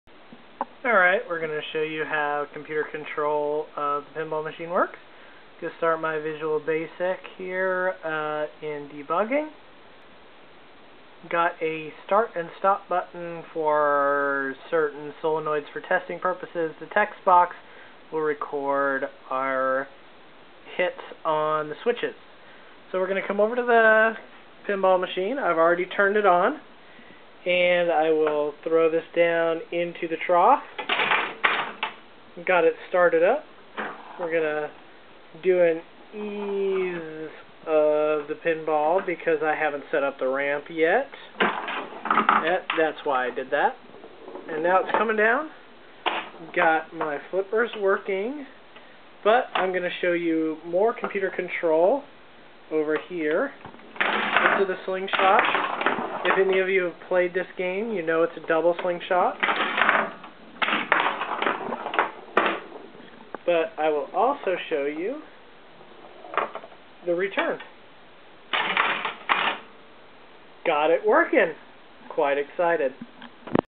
Pinball Computer Control
I now have control of my solenoids with vb and a UHID.